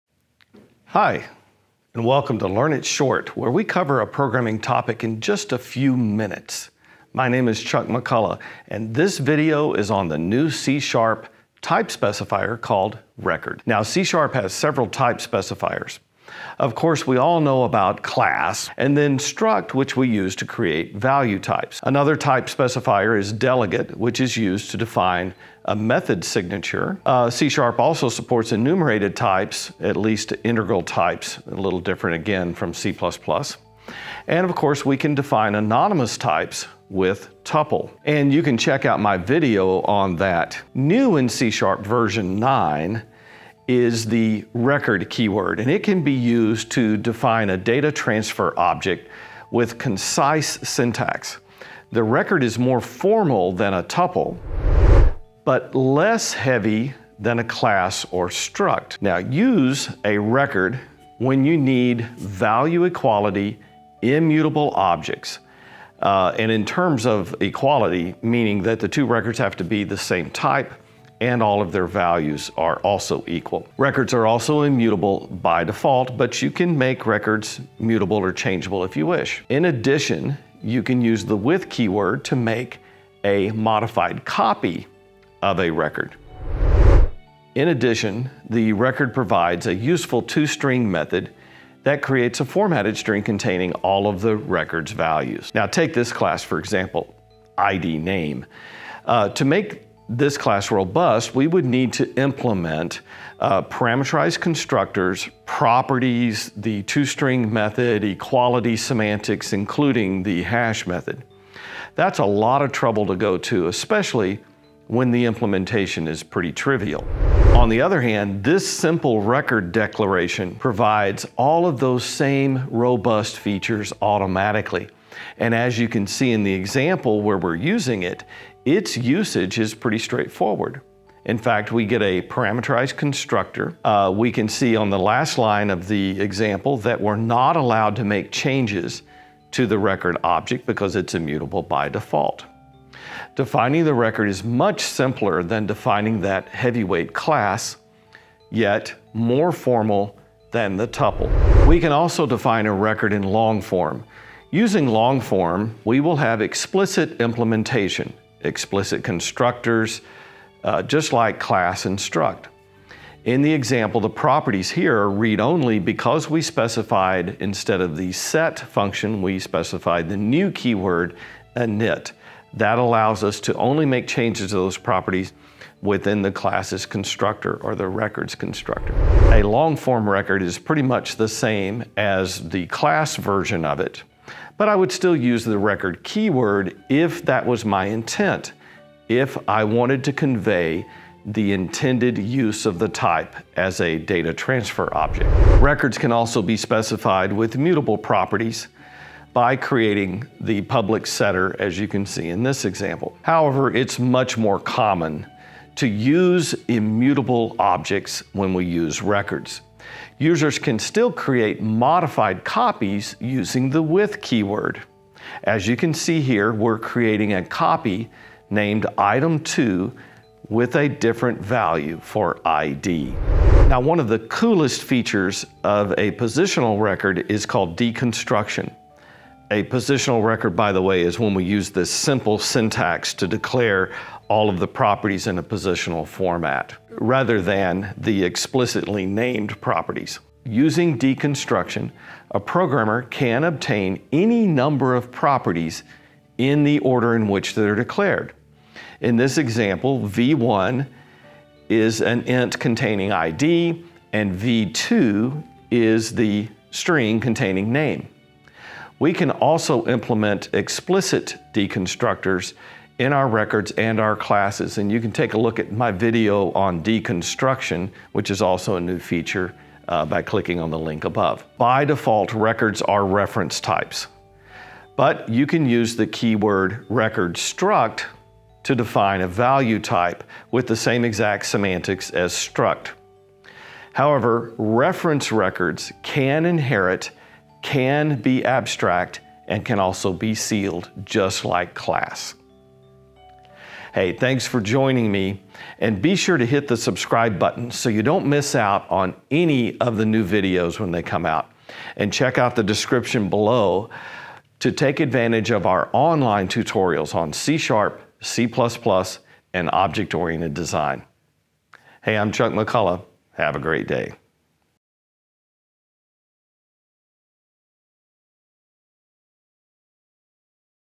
Learn All About Record Types in C#
Chuck explains the new record type specifier in C#.

Check out the other videos on this channel for more details and check out our training courses